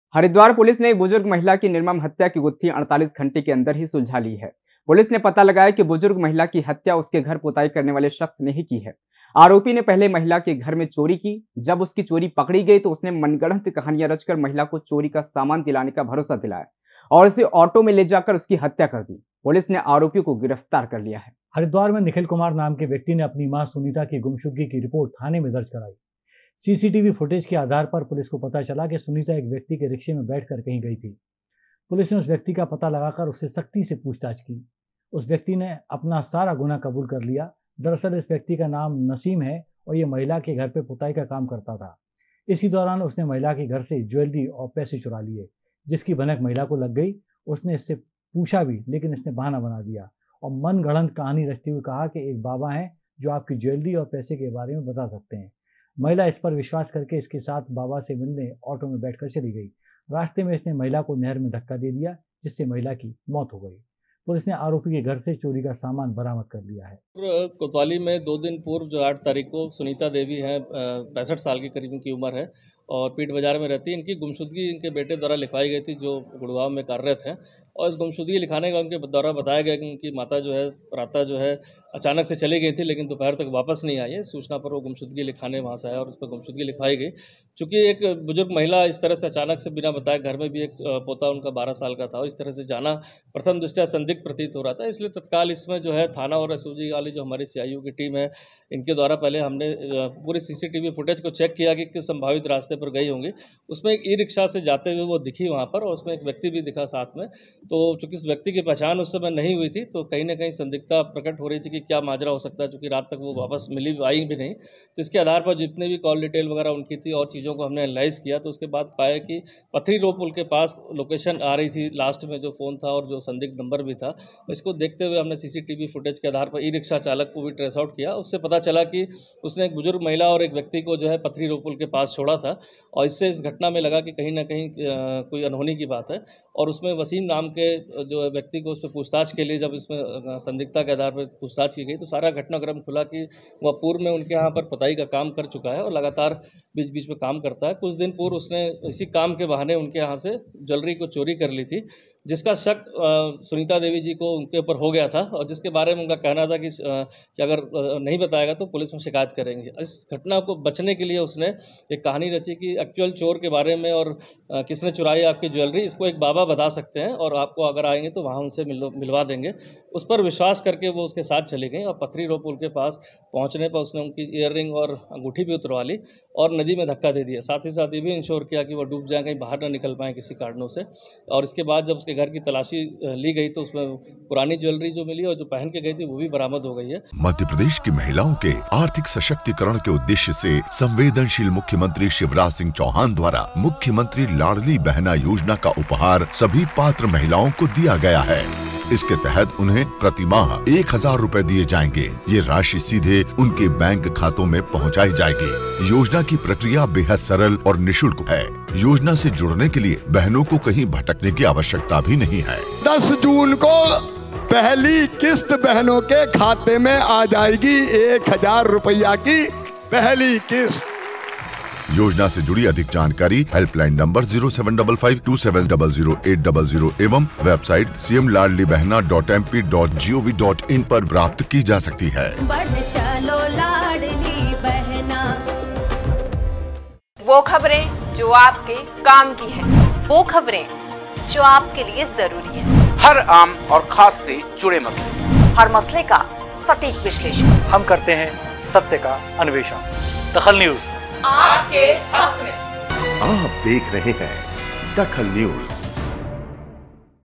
DAKHAL NEWS पुलिस ने सुलझाया बुजुर्ग महिला की हत्या का रहस्य
पुलिस ने सुलझाया बुजुर्ग महिला की हत्या का रहस्य
बुजुर्ग के घर पुताई करने आया शख्स निकला कातिल